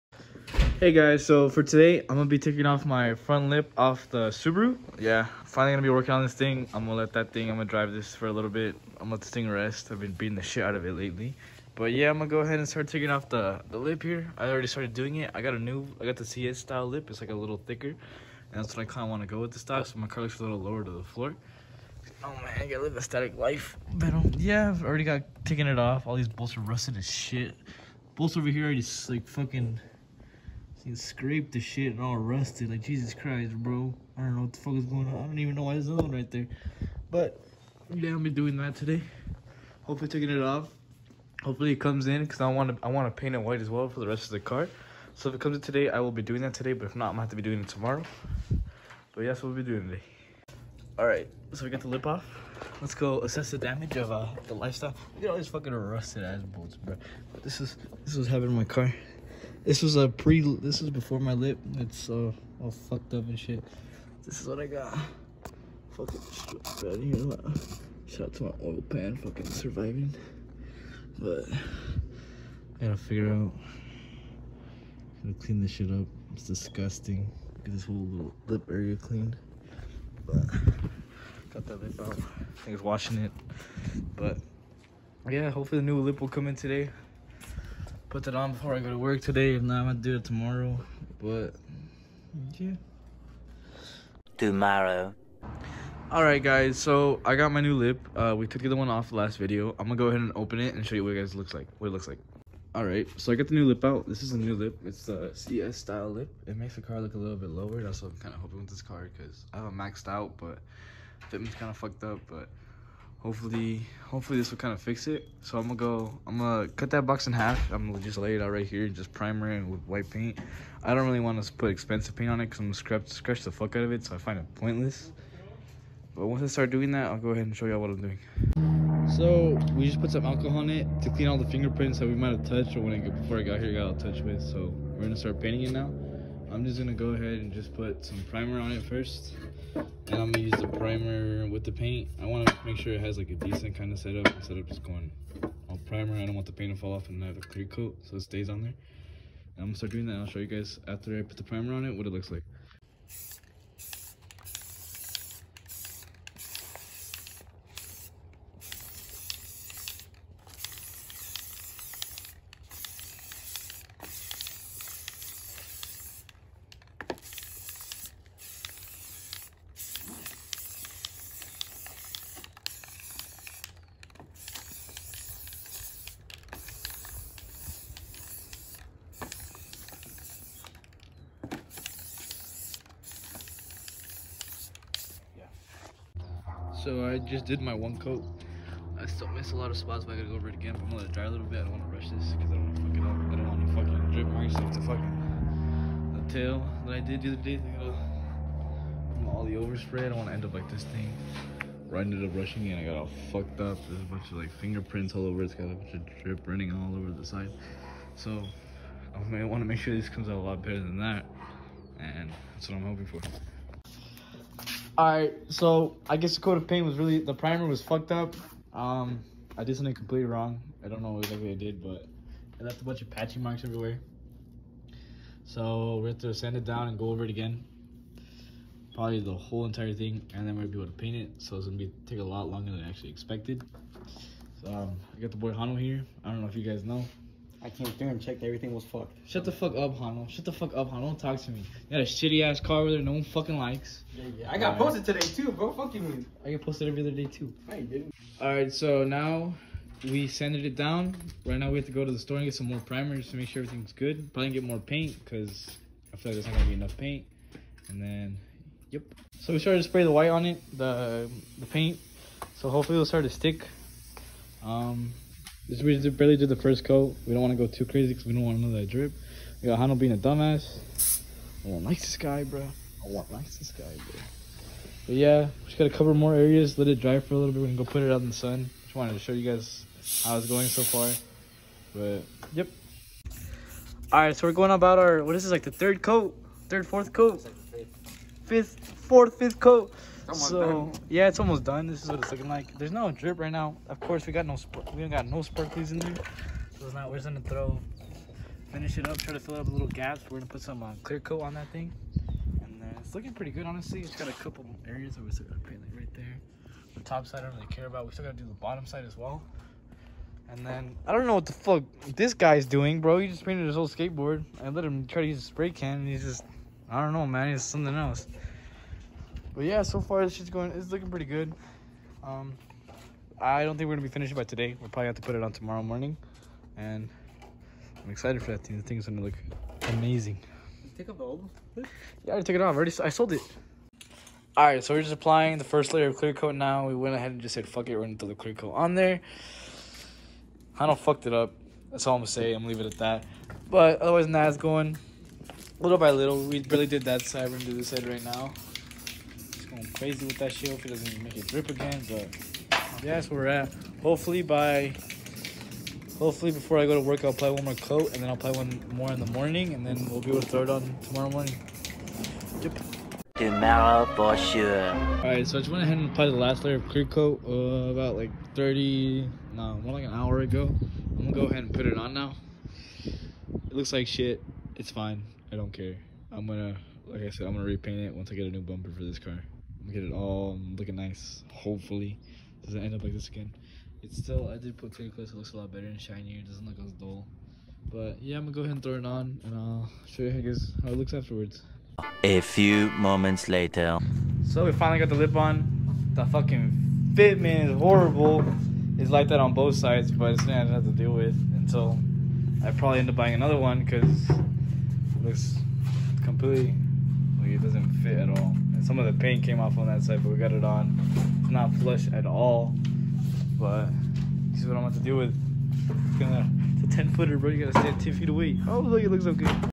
Painting the new lip on my Subaru WRX.
Today we're gonna be painting a new lip ( CS Style ) on my Subaru WRX.